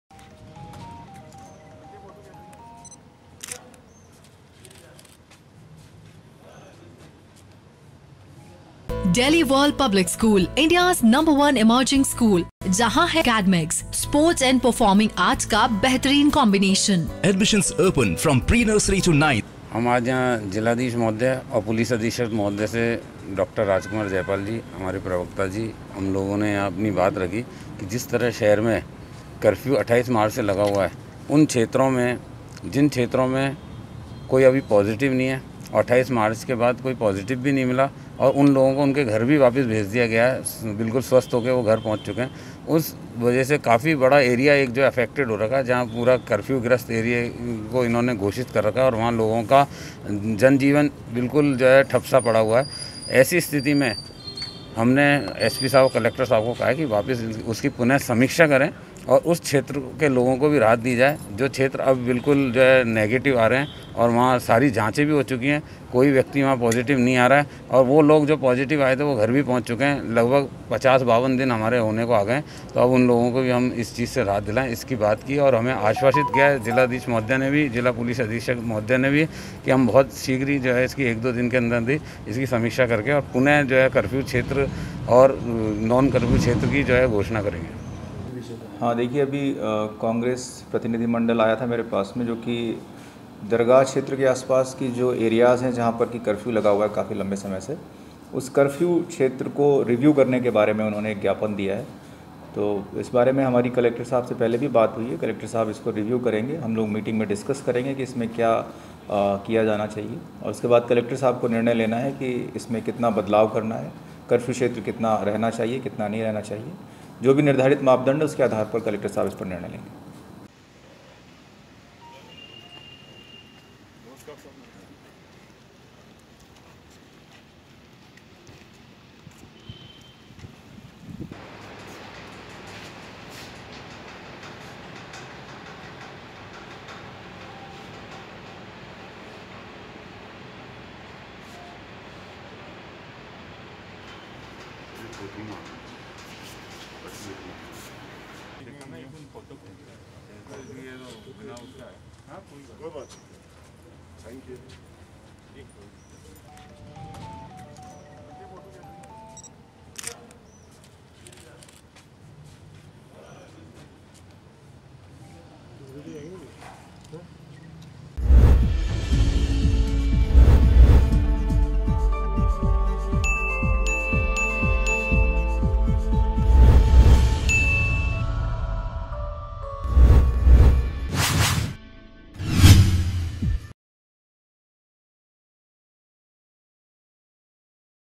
AJMER NEWS | कांग्रेस नेता कर्फ्यू की समीक्षा करने की मांग लेकर पहुँचे कलेक्ट्रेट | MTTV INDIA
कांग्रेस नेता अजमेर के चार थाना क्षेत्रों में लगे कर्फ्यू की समीक्षा करने की मांग लेकर पहुँचे कलेक्ट्रेट
जिला कलेक्टर और पुलिस अधीक्षक से की मांग
शहर कांग्रेस अध्यक्ष विजय जैन और पूर्व विधायक डॉ राजकुमार जयपाल ने की मुलाकात